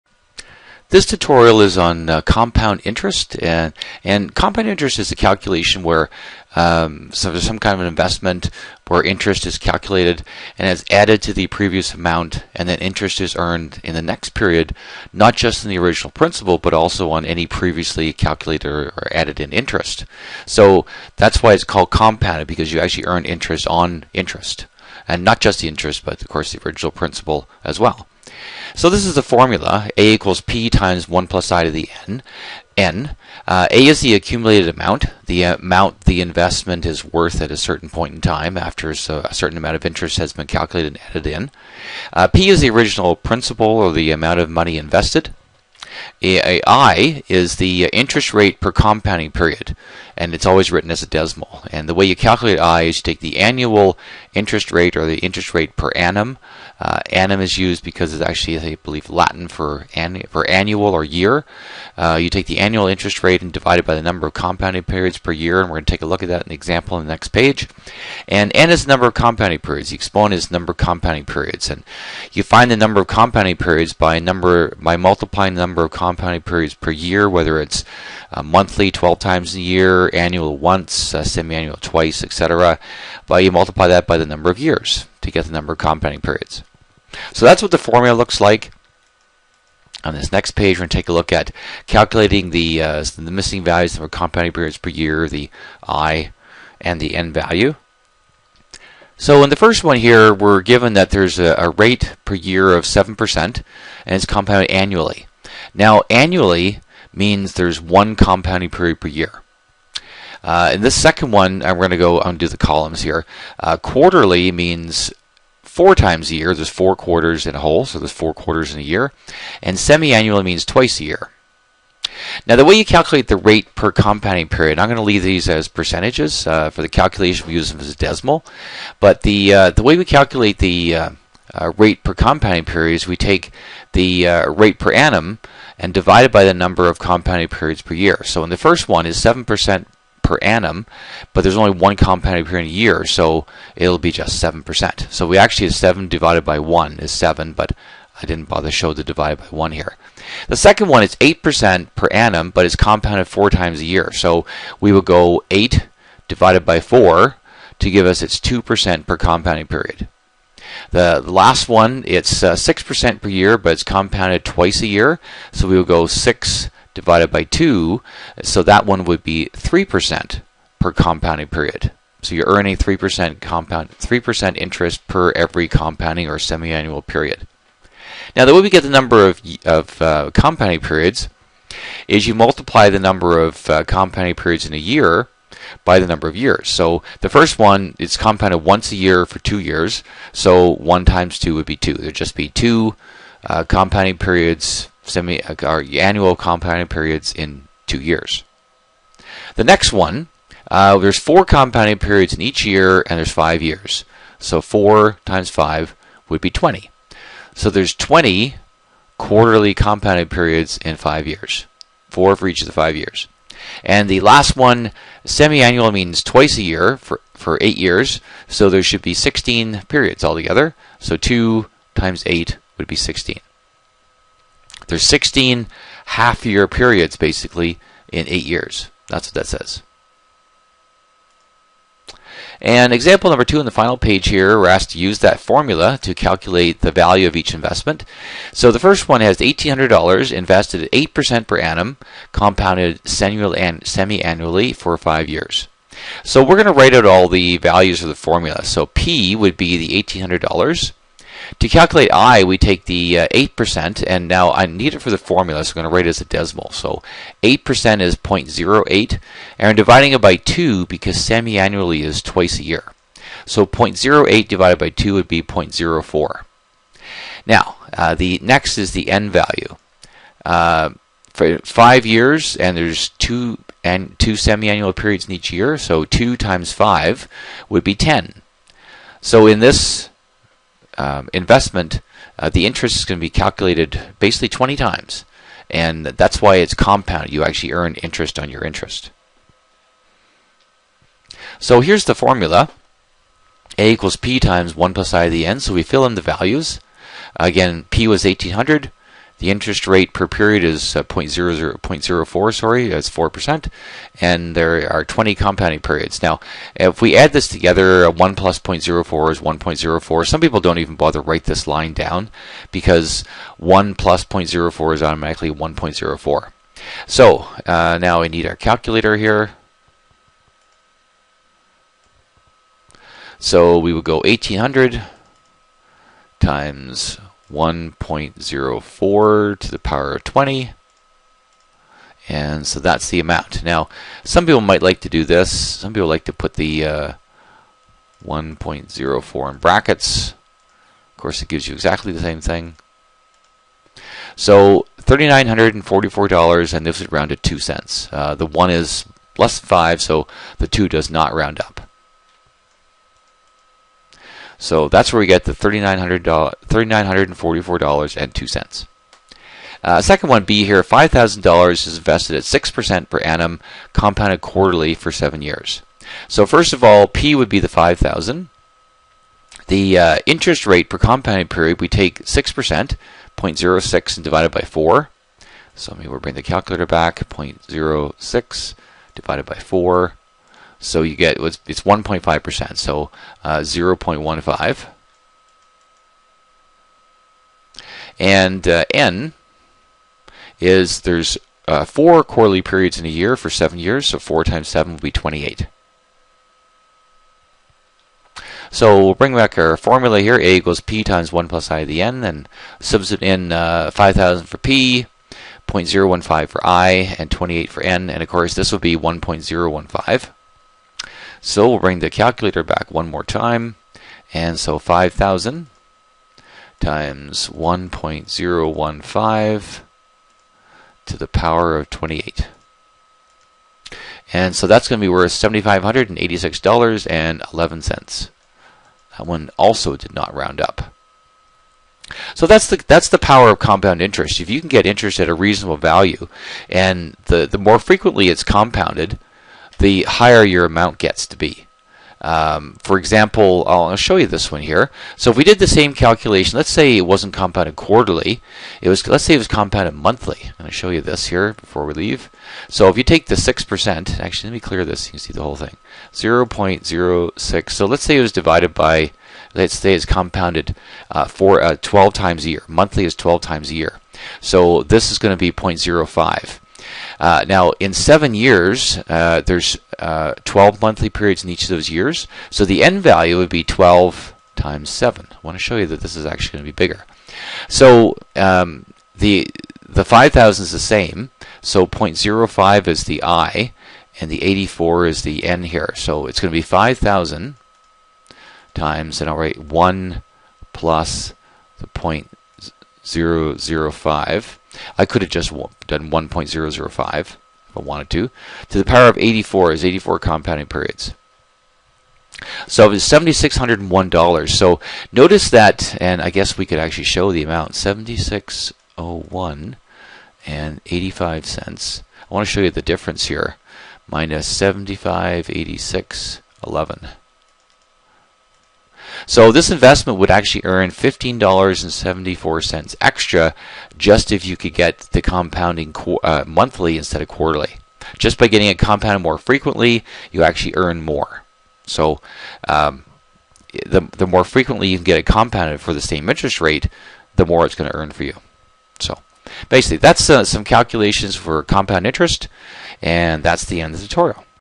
Compound Interest
This tutorial was created for the Workplace Math course (MEL3E) in the province of Ontario, Canada. This video explains how to calculate compound interest using A = P(1+i)^n.  An example also includes how to determine i (interest rate per compounding period) and n (the number of compounding periods).